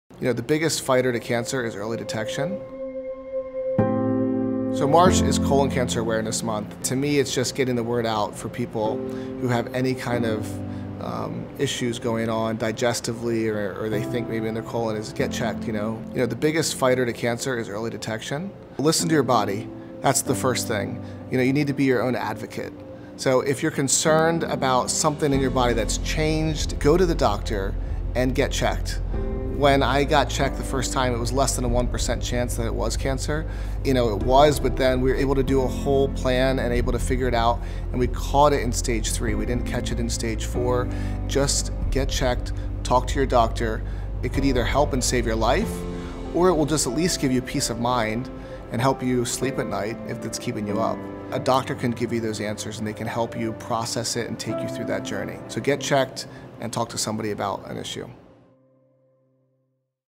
The Days After | Check Yourself - Miniseries
"Today, one year after my last chemo treatment, I celebrate my life and my health. In honor of this momentous anniversary and National Colorectal Cancer Awareness Month, I challenge you to be your own health advocate." - Derek Dienner, Cancer Survivor

Thank you to LUCA for allowing us to film in your space!